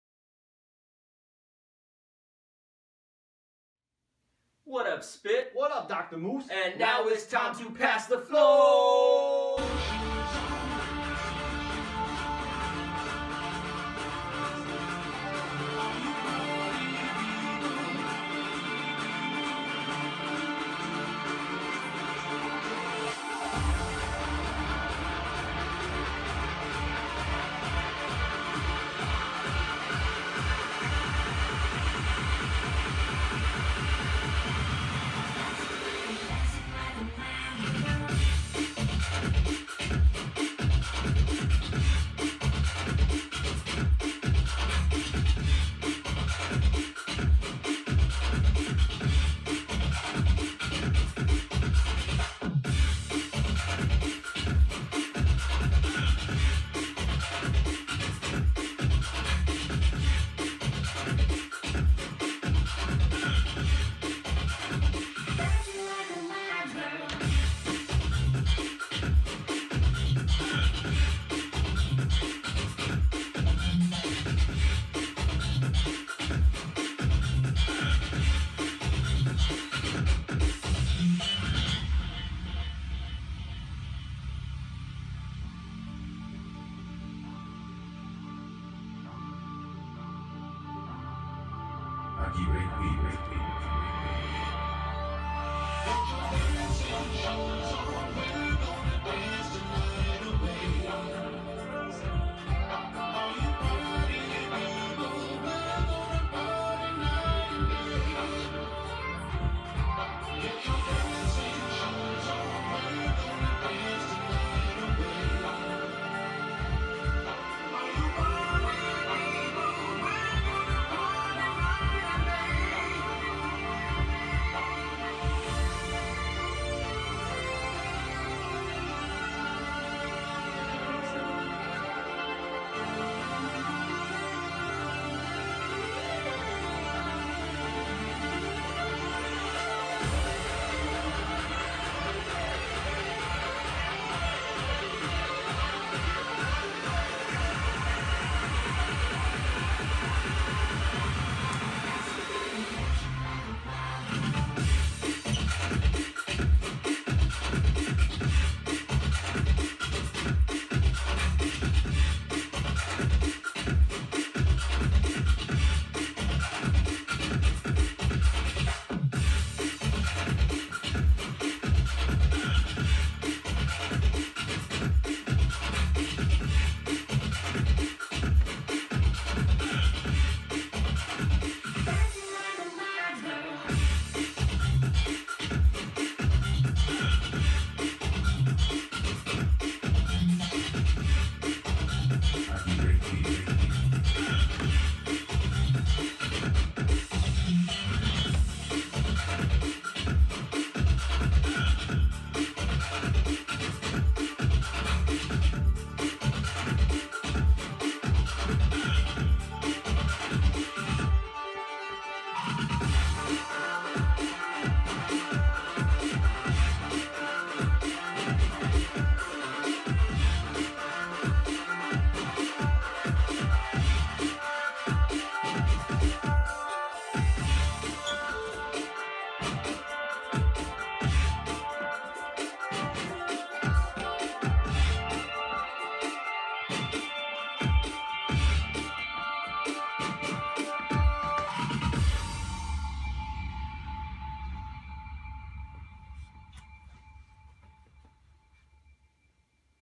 Moose and Spit Pass The Flow
Dr. Moose and B-boy Spit passing the flow back and forth.